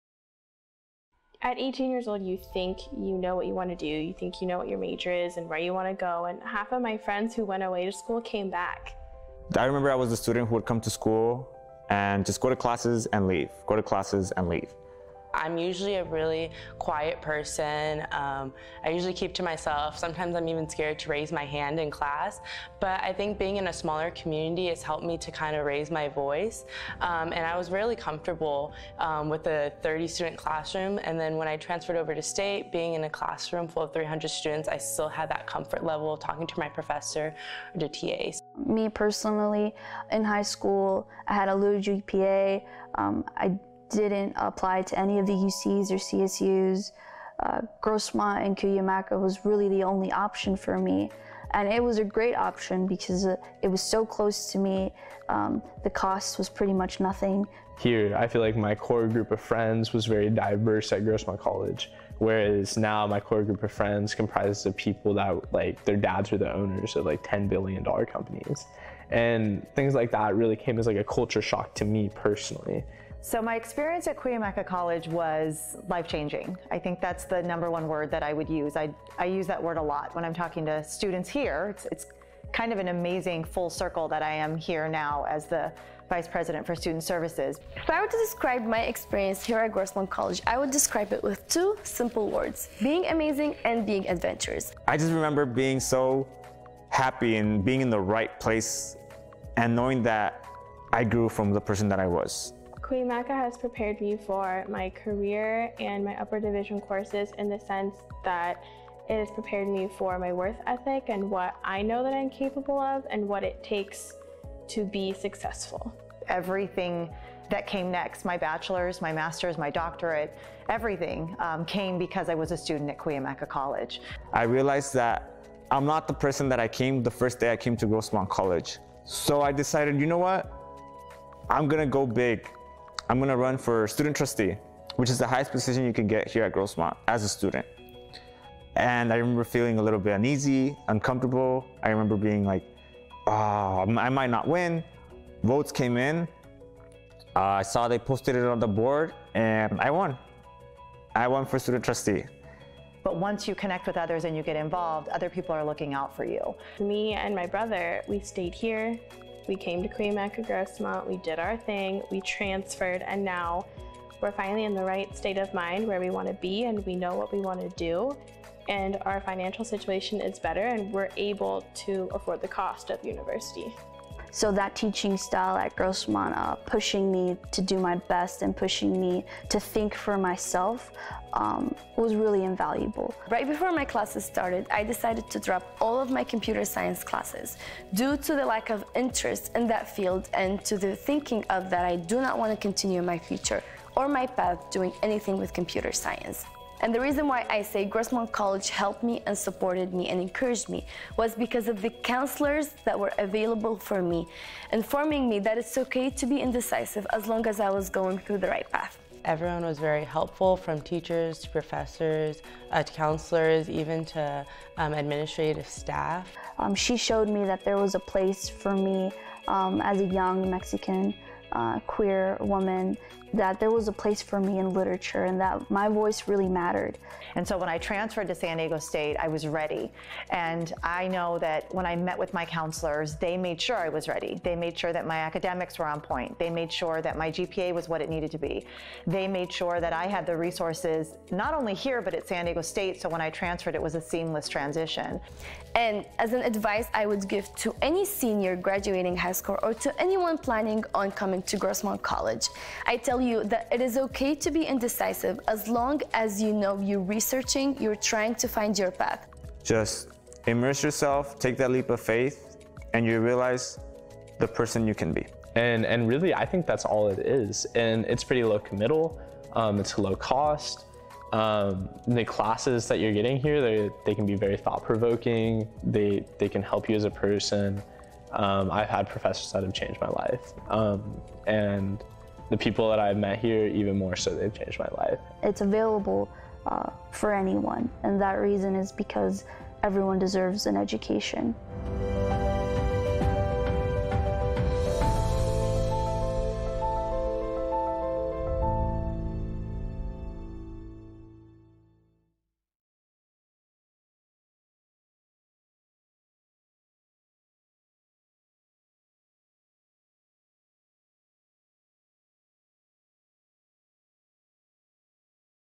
Student Success Stories | Grossmont & Cuyamaca Colleges
Successful and inspirational stories of various students who graduated from Grossmont and Cuyamaca Colleges and transferred to Universities across California.

 Special thanks to all of the participants, whose info is displayed at the end of this video.

Video Created by : Grossmont Outreach